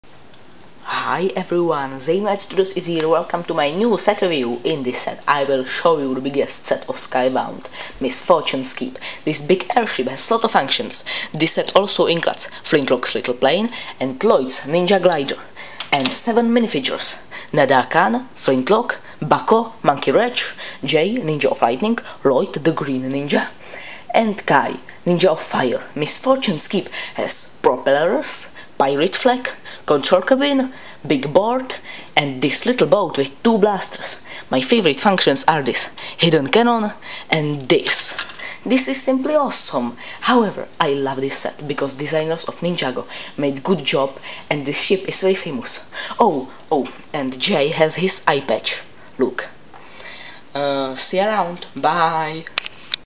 NADAKHAN'S (AIR)SHIP | Lego Ninjago 70605 Misfortune's keep set review
Hui everyone, this is Misfortune's keep set review. Hope u like it.